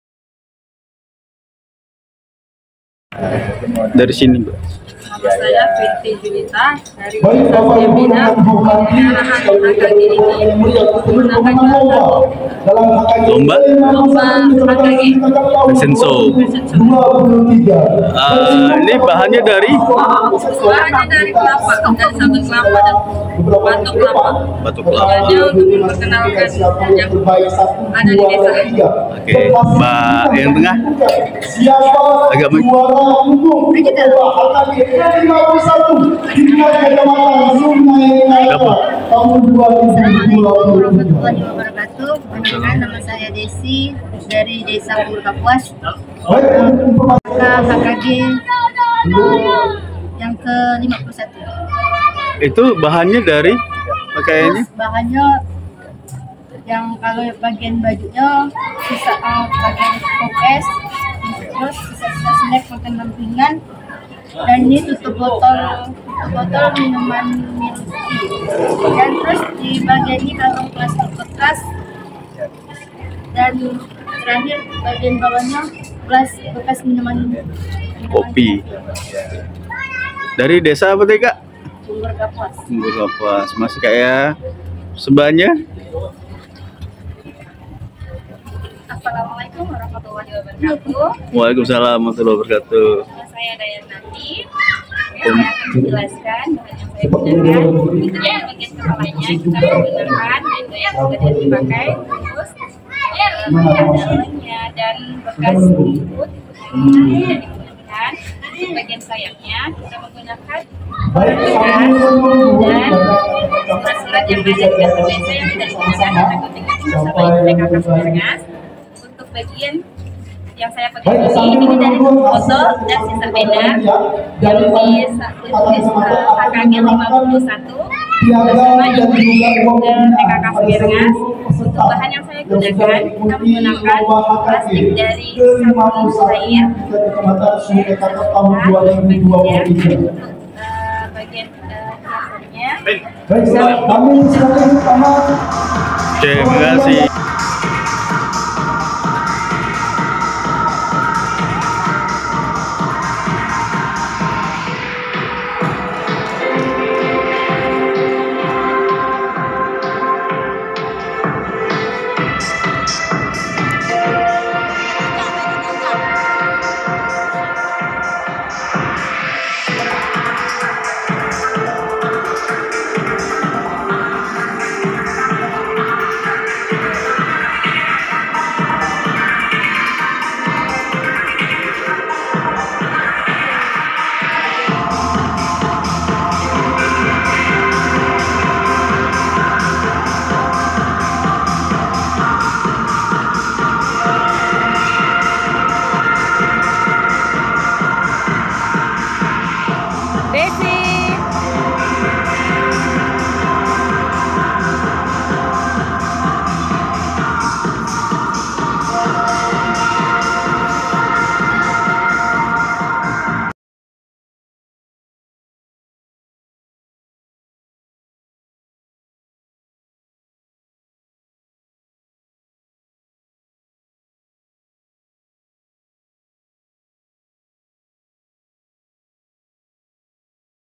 Lomba Fasion Show bahan daur ulang
🌟 Saksikan keseruan Lomba Fashion Show Bahan Daur Ulang yang diselenggarakan oleh TP PKK Kecamatan Sungai Kakap, Dalam acara ini, kreativitas para peserta diuji dengan menggunakan bahan daur ulang sebagai bahan utama pakaian mereka. 💚
👗 Dari sampah yang tidak terpakai, para peserta berhasil menciptakan karya fashion yang menakjubkan dan penuh inspirasi. Video ini bukan hanya menunjukkan bakat, tapi juga mengajak kita untuk lebih peduli terhadap lingkungan dengan memanfaatkan bahan yang bisa didaur ulang.
✨ Jangan lewatkan momen seru dari acara ini! Klik dan tonton sekarang, dan dukung gerakan PKK HKGPKK BankSampahMajuBahagia BankSampah yang terus mengedukasi kita untuk menjaga bumi agar tetap indah dan bersih!